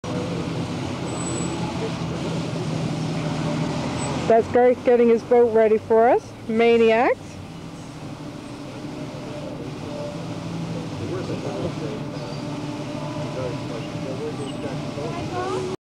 Cousins Sunset 1991 ptA short Garth clip[
Quoting Pamela - Garth getting his boat ready.. maniac !...  HA!
see Cousins Sunset Part B clip for the Birthday party shenanigans